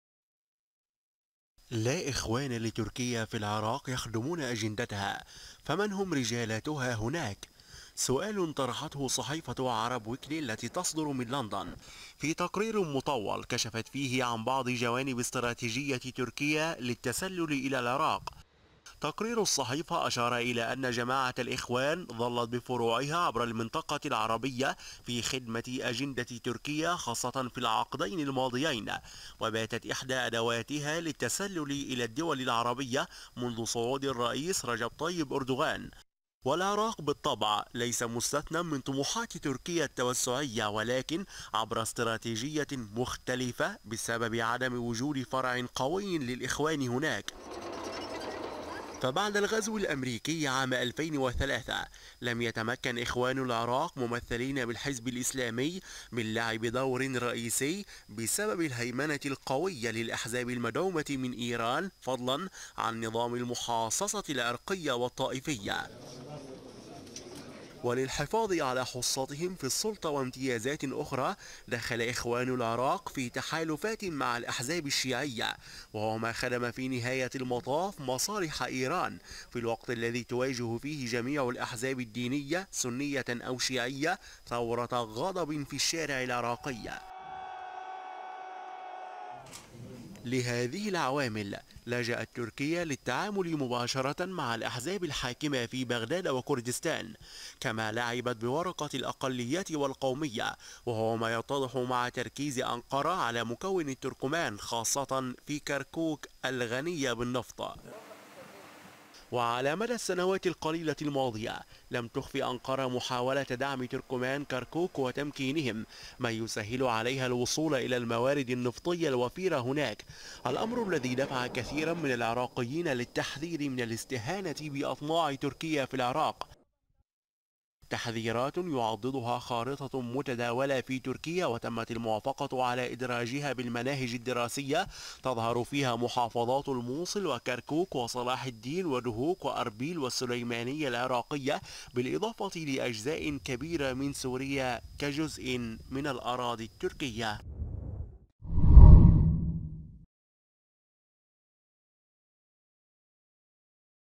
لماذا لم تستعن تركيا بالإخوان خلال تدخلها بالعراق؟
رغم استعانة تركيا بجماعة الإخوان في عدة دول عربية، لتحقيق مخططها التوسعي؛ إلا أنها لم تستعن بتلك الجماعة في العراق، وبحسب صحيفة عرب ويكلي لعبت تركيا على ورقة القومية، للتسلل إلى بلاد الرافدين عبر دعم التركمان العراقيين.
ويقول مراقبون، إن زيادة التدخل العسكري التركي في العراق تحت ذريعة مكافحة الإرهاب، من المُحتمل أن يكون وسيلة لاختبار ردود الفعل المحلية والإقليمية والدولية قبل الغزو الفعلي والاحتلال المستقبلي لأراضٍ عراقية.
المزيد من التفاصيل في سياق تقرير صابر سعد.